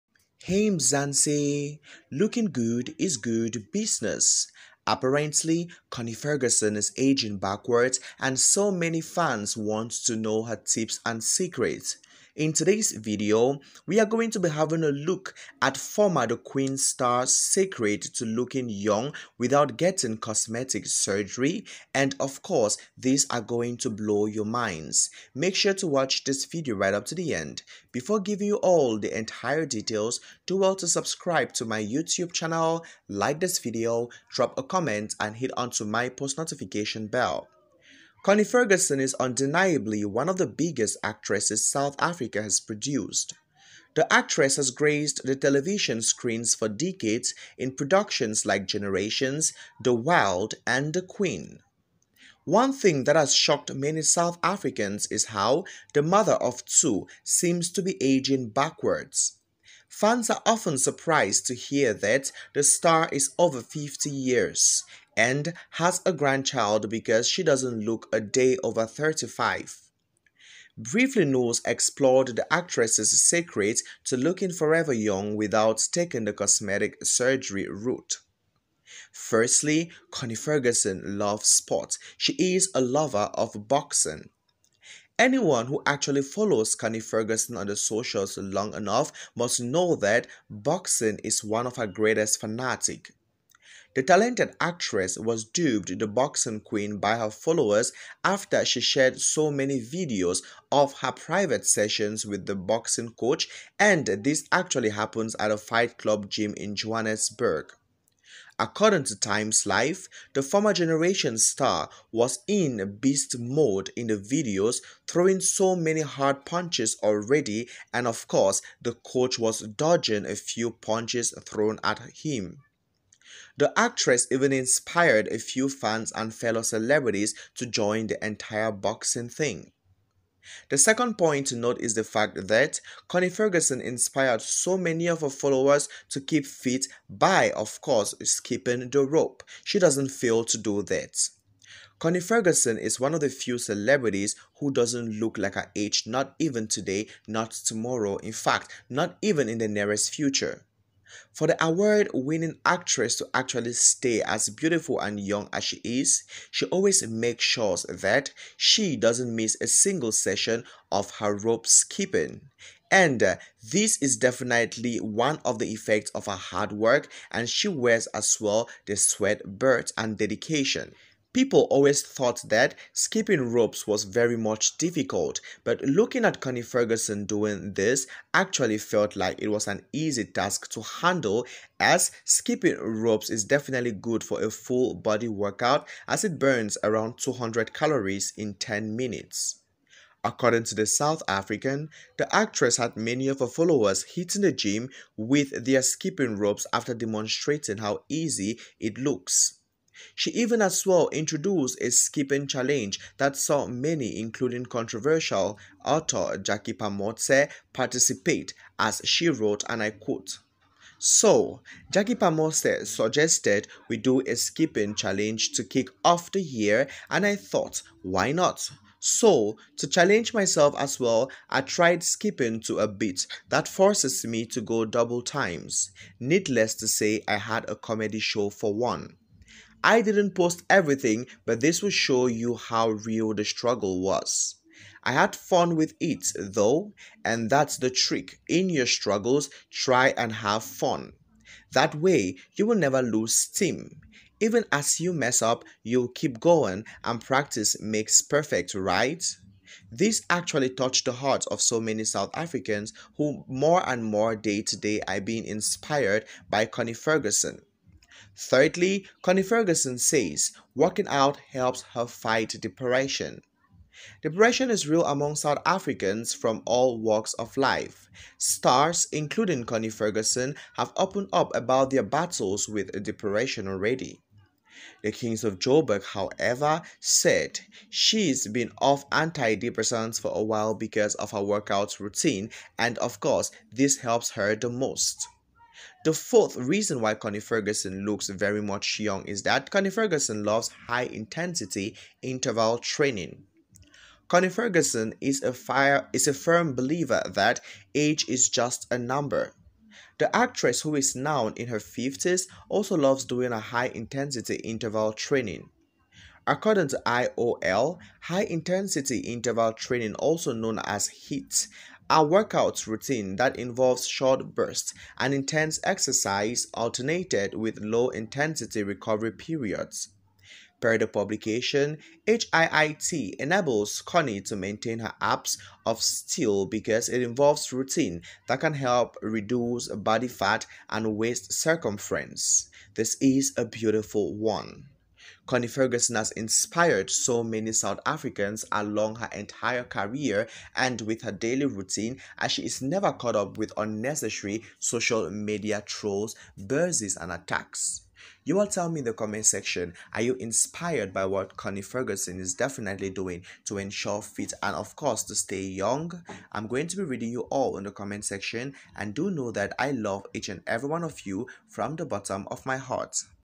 Connie Ferguson Secret activities are Revealed in full video, you will be Shocked why she ages less
Connie Ferguson Secret activities are Revealed in full video, you will be Shocked